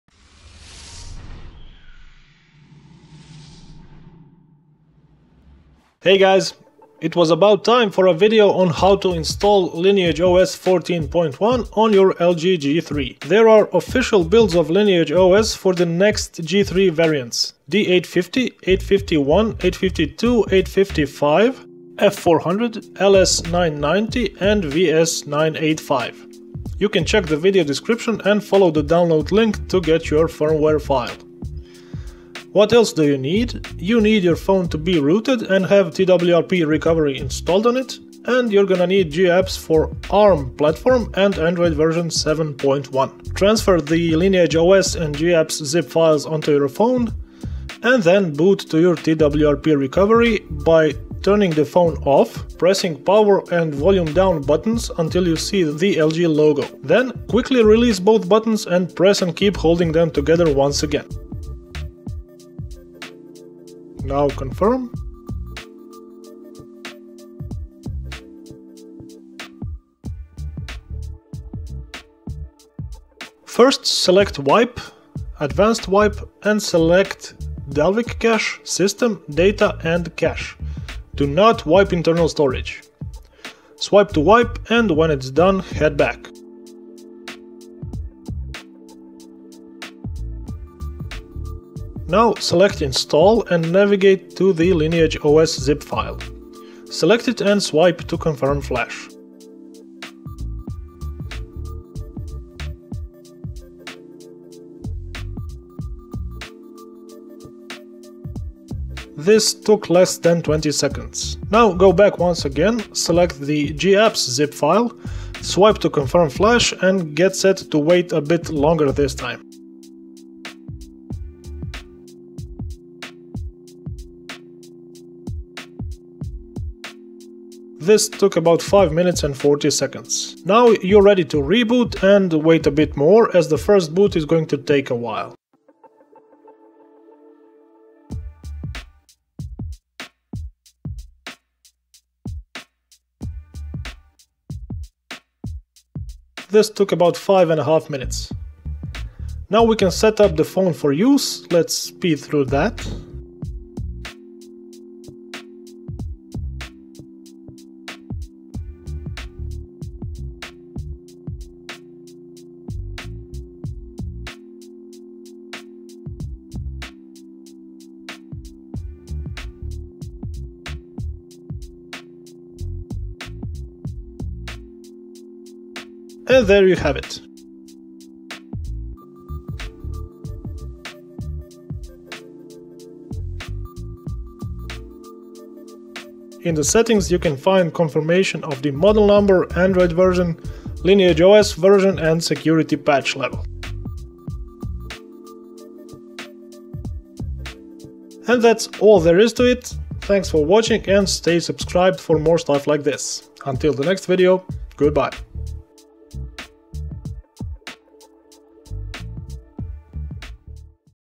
How to install Official Lineage OS 14.1 on LG G3 - Android 7.1.2 Nougat [Tutorial]
(Note: I recommend NOT flashing SuperSU when prompted in TWRP. Flashing it may result in the ROM not booting.)
In today's short tutorial you'll find out how to install Official Lineage OS 14.1 on LG G3. The device used in the video is the unlocked, D855 version.

Credit to the developers.

Remember: anything you do to your phone or on your phone is your own responsibility. ;-)